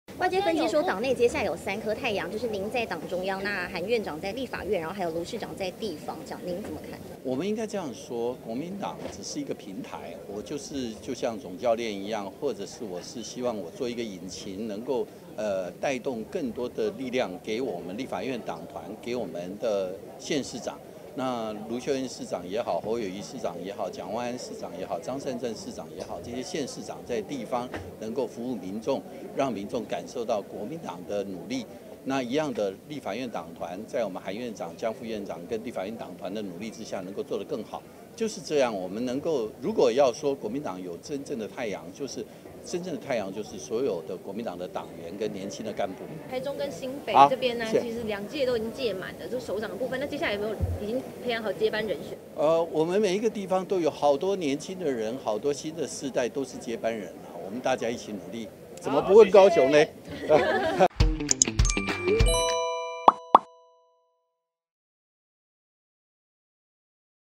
與韓國瑜、盧秀燕列國民黨3個太陽？朱立倫：我希望做一個引擎【CNEWS】
CNEWS匯流新聞網記者陳儀潔、 韓羽婕、王聿瀠、李文成、林佑威、施建宏／台北報導